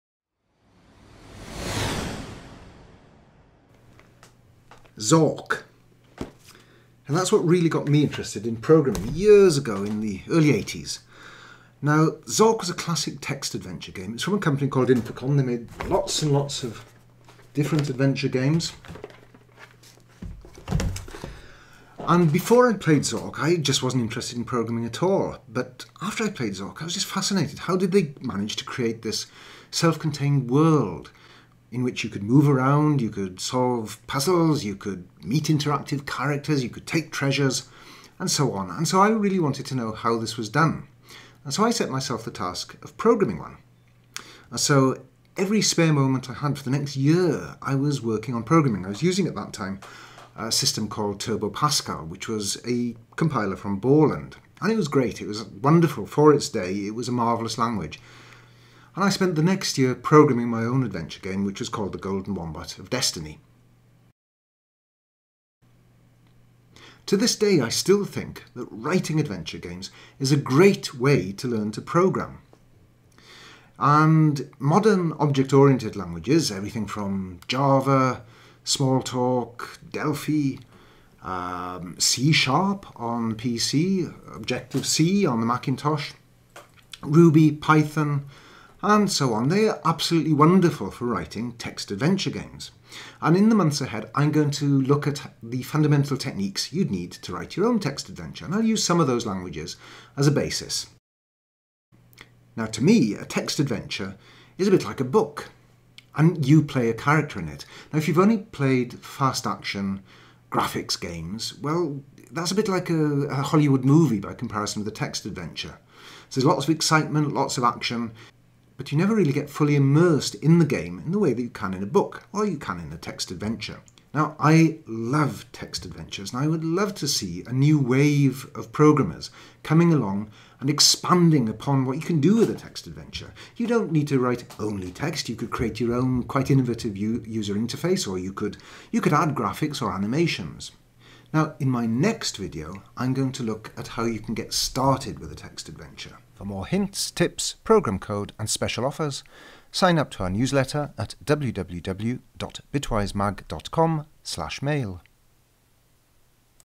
Learn to program adventure games
Learn To Program Learn to program by writing adventure games. In this introductory video, I give a bit of background on text adventures. In future videos, I'll explain how to write your own games in various different programming languages.

For more information on SapphireSteel Software's programming tools go to the SapphireSteel Software web